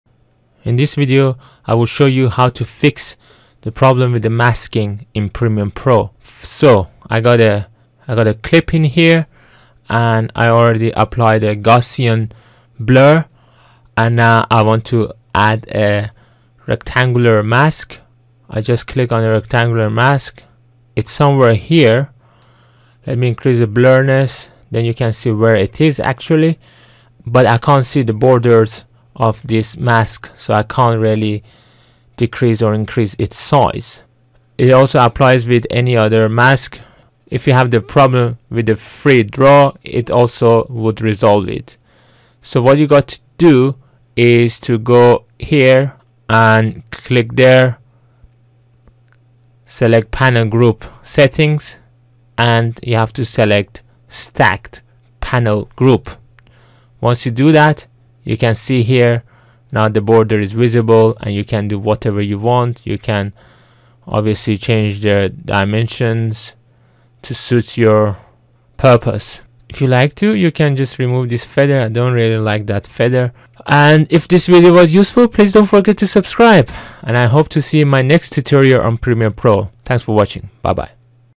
Solved I can't see MASKS edges in Premiere Pro
There are lot of things that can wrong with Premiere Pro and one of them is when you can't see the borders of the masks tool. I was trying to blur a portion of my screen that I realized the missing border. Luckily, for every problem in premiere pro, there is a solution. And many times the solution is simpler than you could imagine.